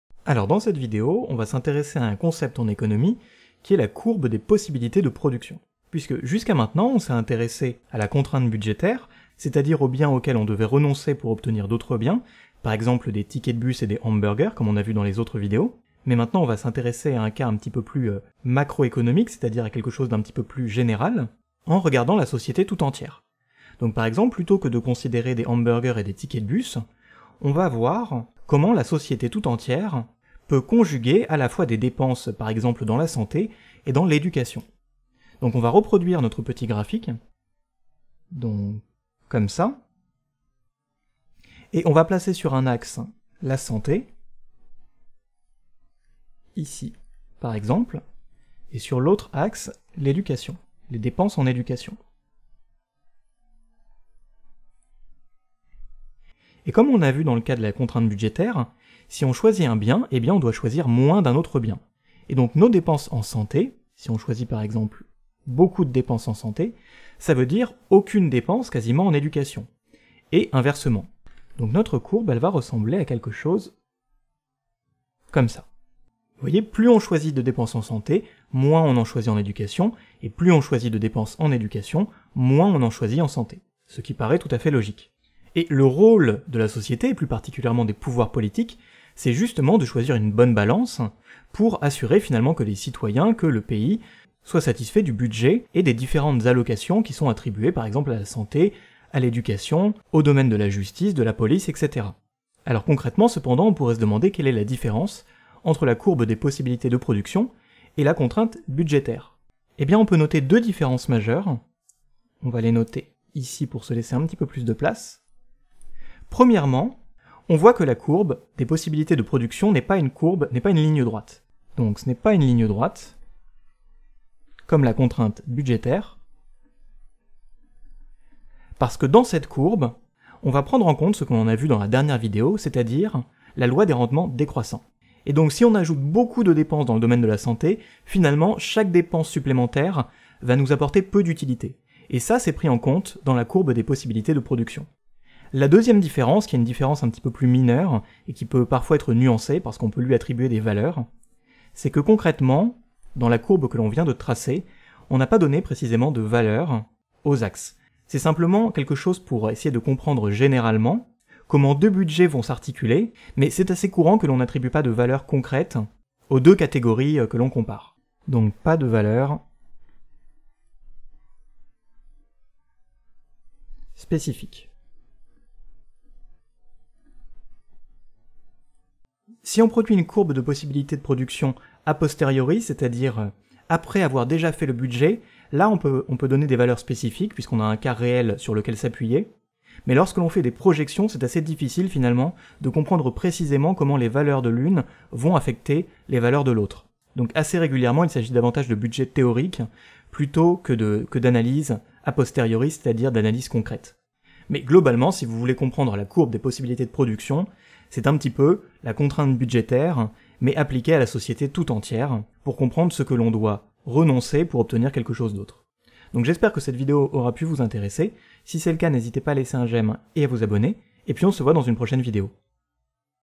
Économie - La courbe des possibilités de production
Retrouvez-moi en live sur Twitch Dans cette vidéo nous allons étudier ce qu'est la courbe des possibilités de production en économie.

Table des matières:
0:00 - Présentation du concept
1:52 - Les différences avec la contrainte budgétaire